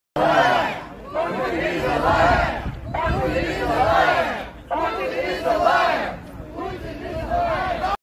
2022.02.25. Dühös tüntetők várták a orosz követet a dublini nagykövetség előtt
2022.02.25. Dühös tüntetők várták a orosz követet a dublini nagykövetség előtt